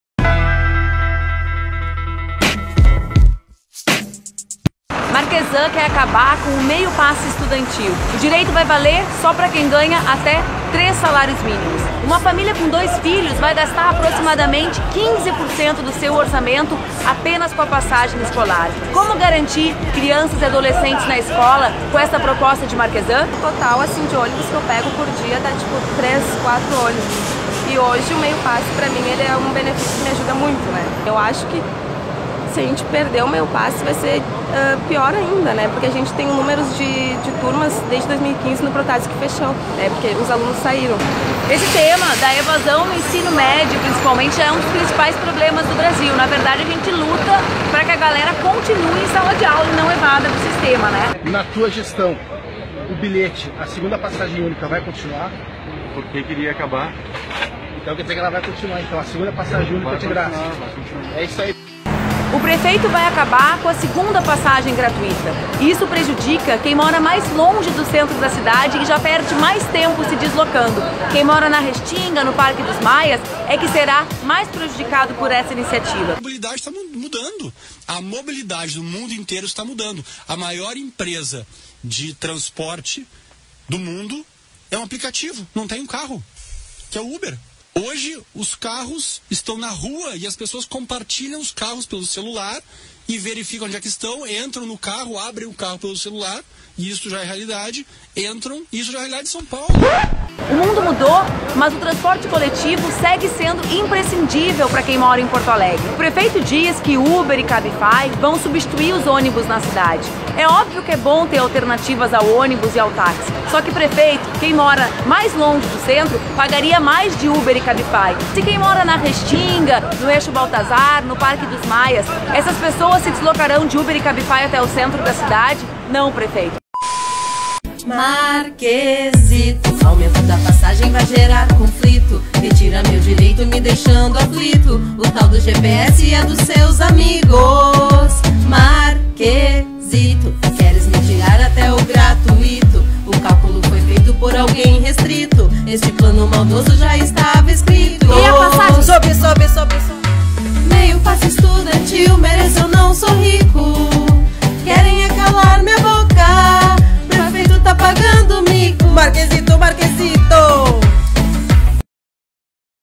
Marchezan quer acabar com o transporte de Porto Alegre!
O prefeito quer acabar com o transporte público de Porto Alegre, confira o nosso recado pra ele neste vídeo.